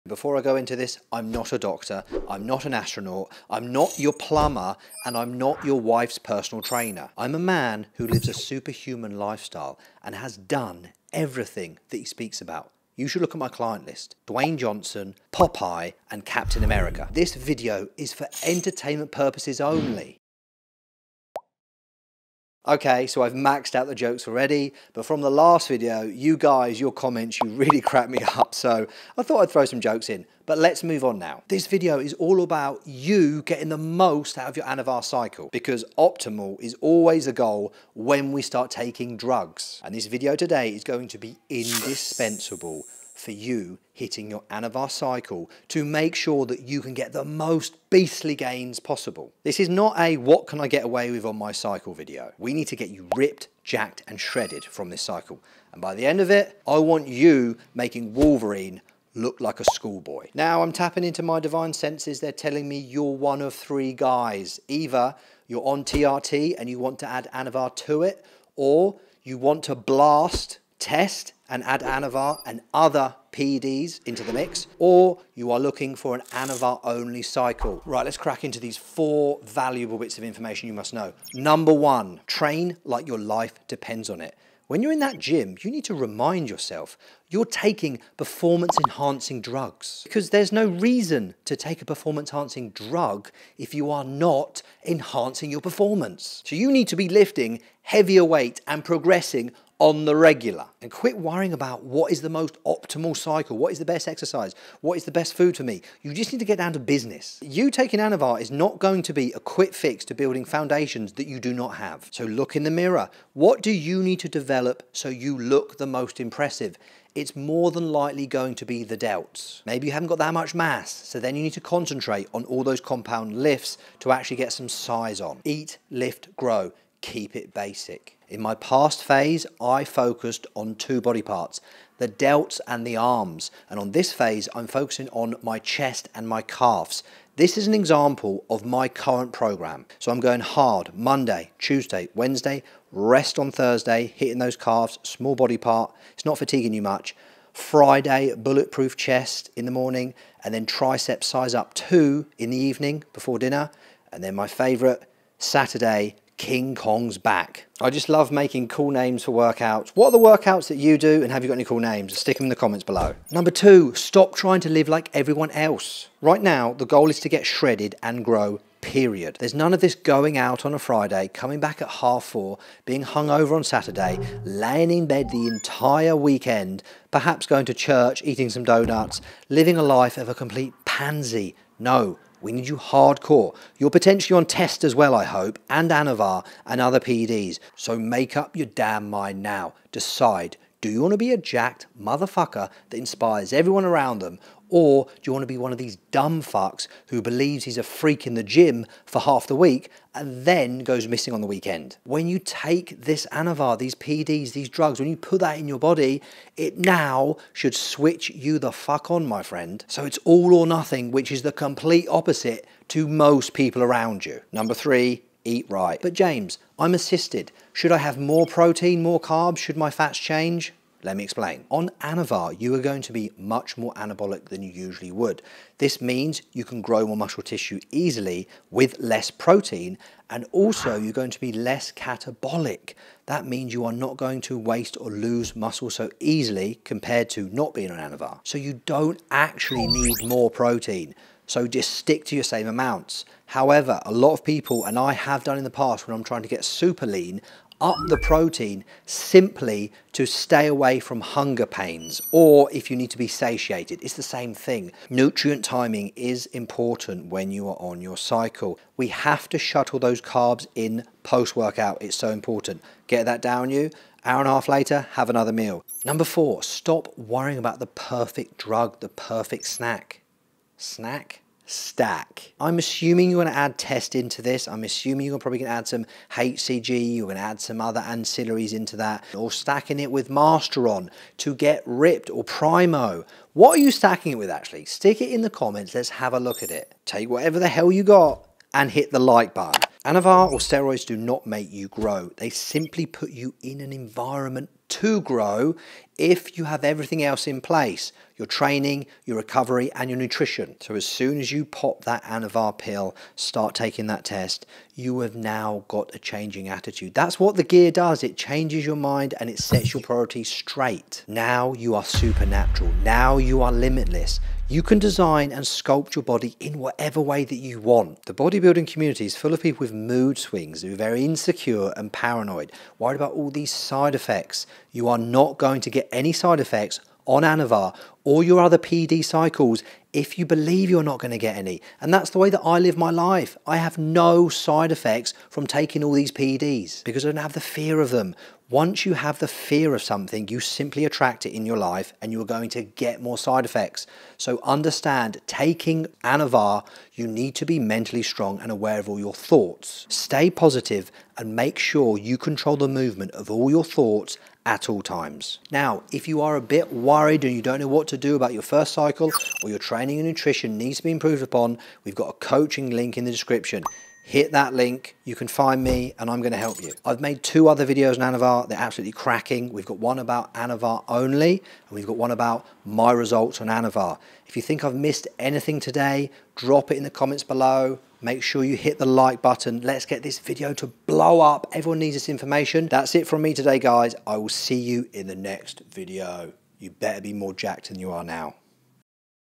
Anavar Cycle - Get The Ultimate Before And After Results!
Will your Anavar Cycle get you the crazy before and after results you want? This video will explain how to get the best out of your Anavar Cycle. Whether you are running an Anavar only Cycle, jumping on Anavar from TRT, or doing a higher Test and other steroids with Anavar, the principles in the video will be indispensable in you leveling up your physique turning you into a real BEAST!

🏆 Need professional guidance on your road to Level Up? 💉 Or Perhaps your blood work reviewed? 🧪

TIME STAMPS:
00.00 Intro: Anavar Only Cycle
01:05 Is this You?
01:21 Tip One: Your Life Depends On This!
02:42 Tip Two: Average Gets You Average
03:34 Tip Three: How To Eat Right On Anavar
04:26 Tip Four: What is The Perfect Anavar Cycle?
05:05 What Does Anavar Do?
05:34 I have no Side effects

Do NOT take steroids. I am NOT A DOCTOR. This video is for fun and entertainment purposes ONLY.